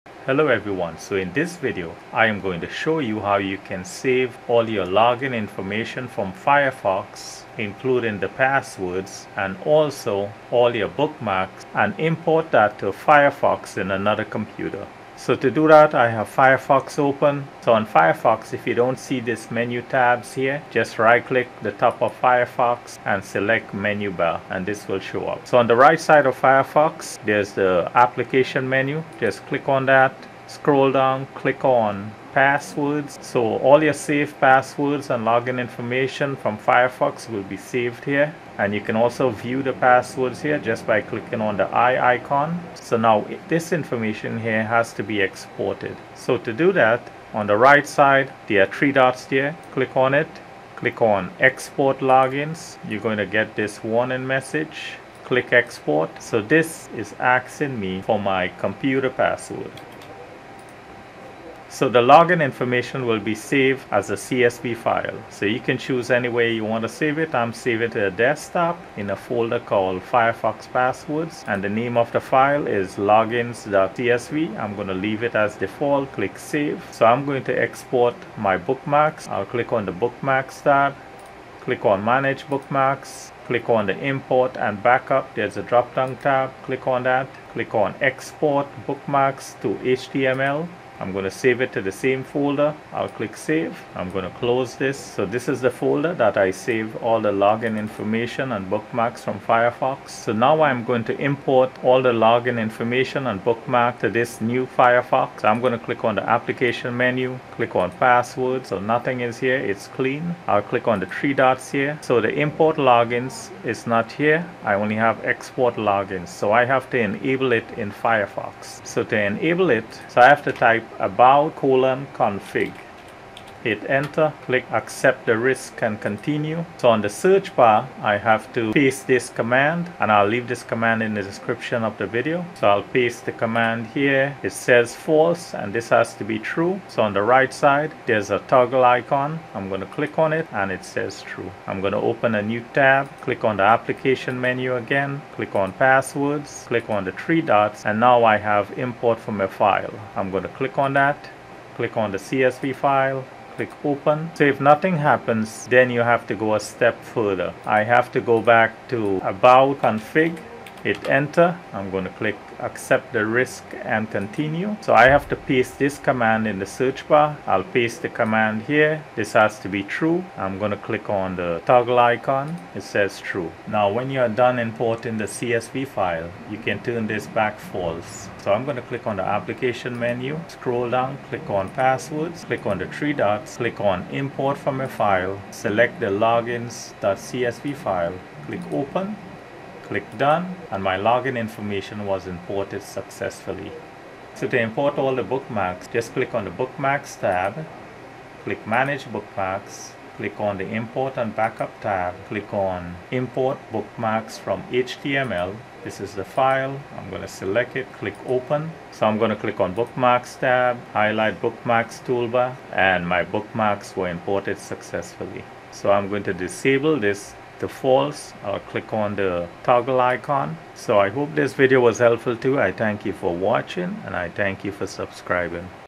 How to export and import saved passwords and bookmarks in Firefox
In this video, I show you how to export/import logins and bookmarks to in Firefox. You can save all your login info and bookmarks in Firefox and transfer it to Firefox in another computer. I show you how to import and export all your saved login info in Firefox.

1) about:config
2) Enable Import:  signon.management.page.fileImport.enabled
3) Import Fix:  security.allow_eval_in_parent_process